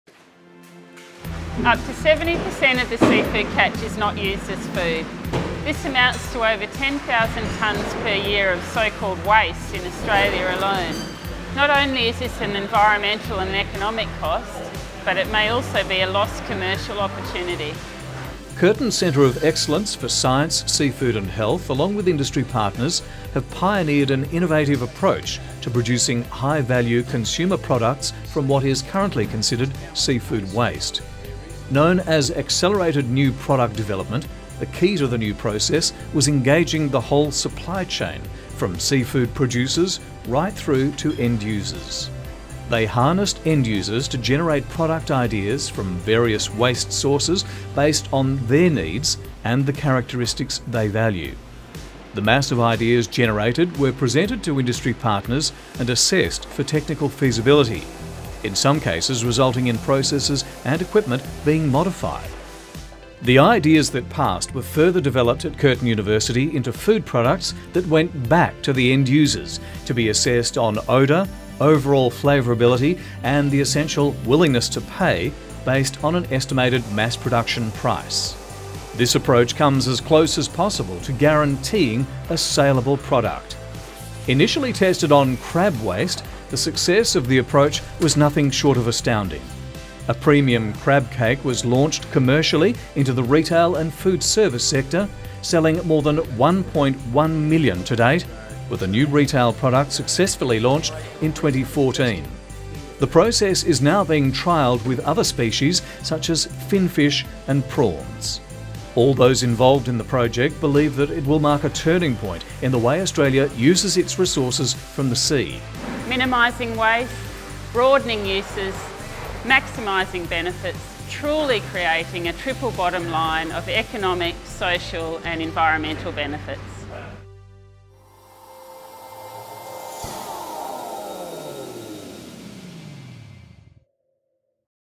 Curtin Rethinks Seafood 'Waste' | Curtin research
For every three fish caught in Australia’s two billion-dollar-a-year seafood industry, the equivalent of two of those are thrown away.
But Curtin University researchers have teamed up with industry partners to reclaim what has to date been a colossal and costly waste.

Watch, and see how this partnership looked outside the box to completely rethink product development.

WINNER - the Health Sciences Prize, supported by the faculty of Health Sciences, Curtin University.

Acknowledgements:
- Peter Jecks: Managing Director, Abacus Fisheries
- Kerri Choo: Supply Chain & Quality Analyst, Geraldton's Fishermen's Co-Operative
- Paul Catalano: Business Manager, Catalano Seafood
- Rachelle Etienne-Breidenbach: Digital Media Manager, Fisheries Research and Development Corporation